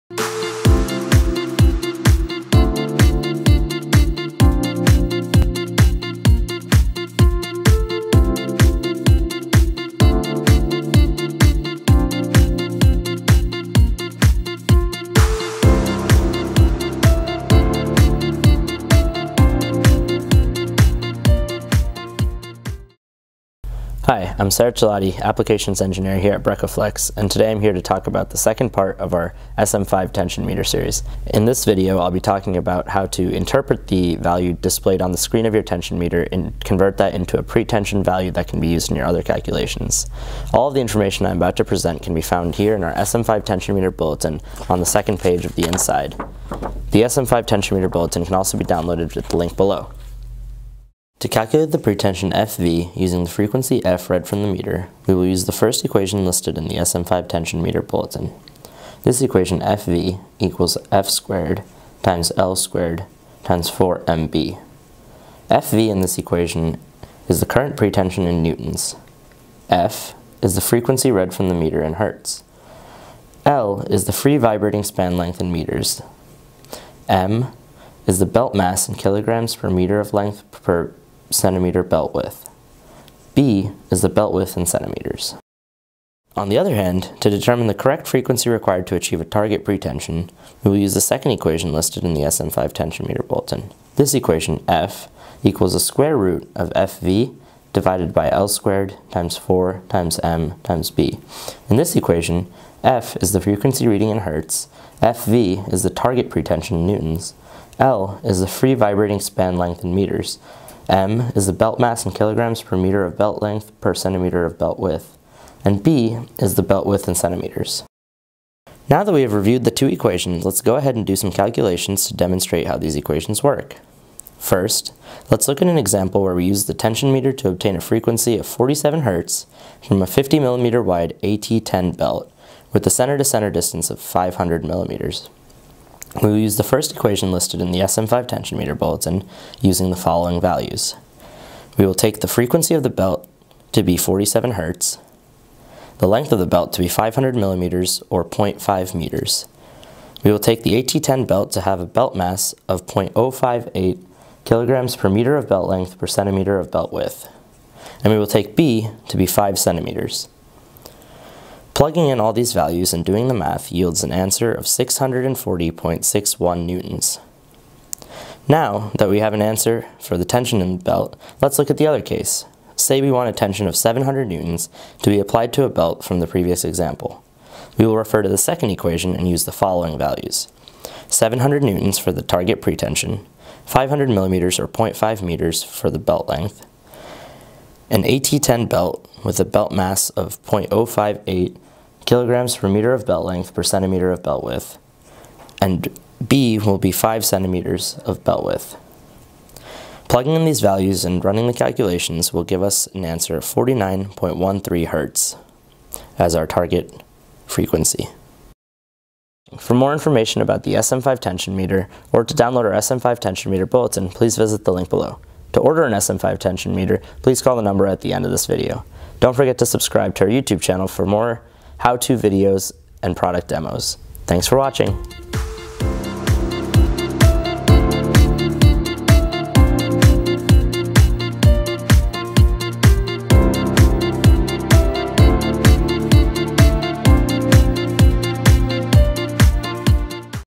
How to Calculate Belt Pretension & Frequency
BRECOflex CO., L.L.C.will show you how to calculate belt pretension from the frequency shown on your SM5 Tension Meter as well as how to calculate frequency from belt tension. All of this information can be found in our SM5 Tension Meter Engineering Bulletin The belt mass for all pitches, both metric and English are located in the table on the inside right page of the bulletin.
For FREE engineering support regarding pre-tension or any belting application, you can speak to Sarath or any of our applications engineers directly by calling the same number and choosing prompt #6